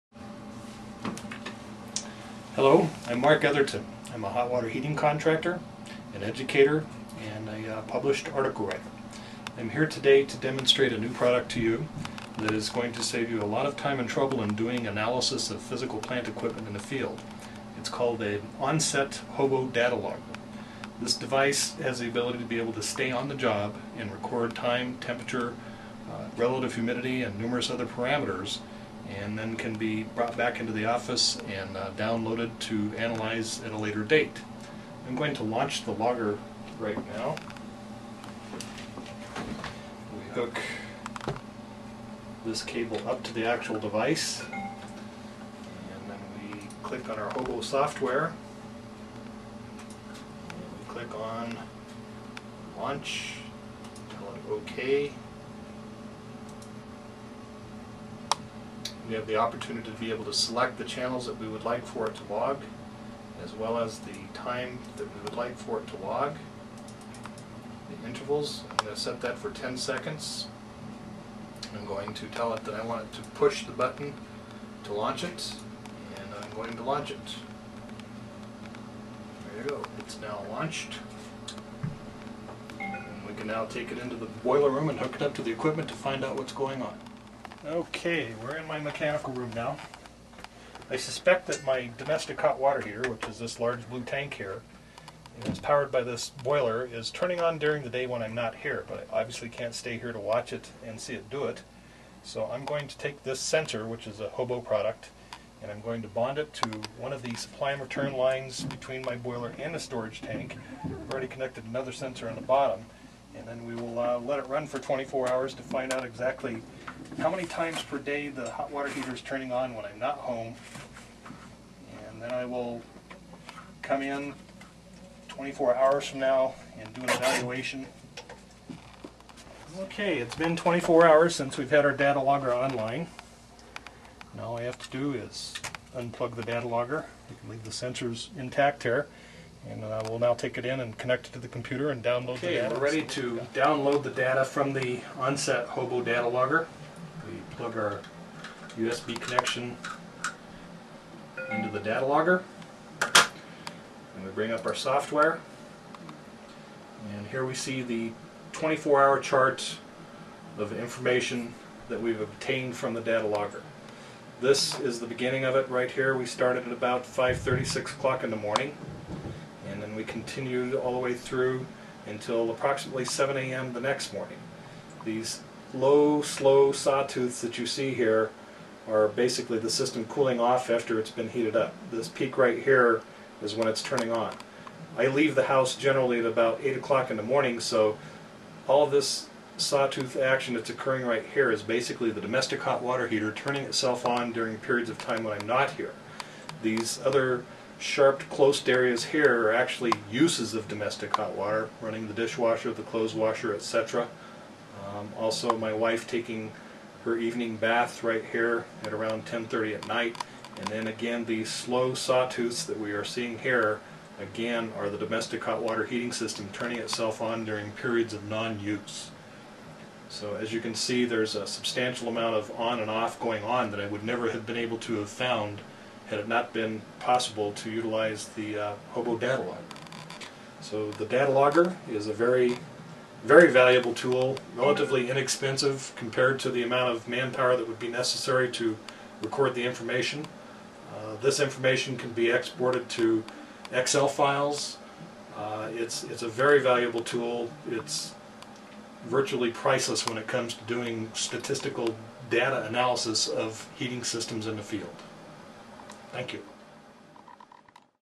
Hot Water Heating Analysis with Data Loggers
This 4-minute "how to" video takes you through the various steps of how HOBO data loggers can help you collect and analyze data on hot water heating system usage.